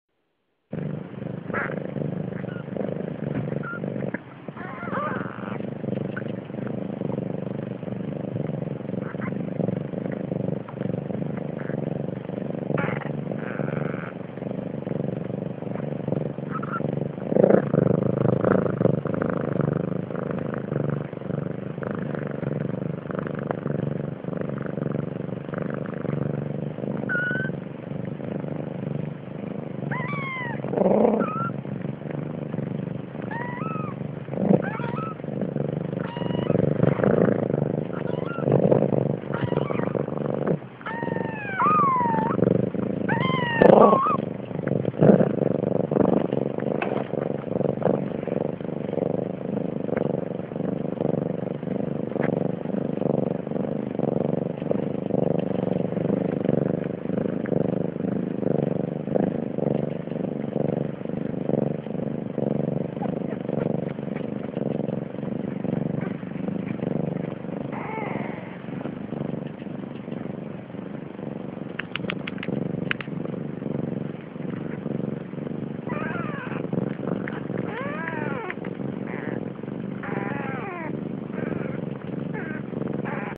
Cute siamese kittens and a protecting mother
Five kittens born 2011 october 30 in Malviken cattery in Sweden.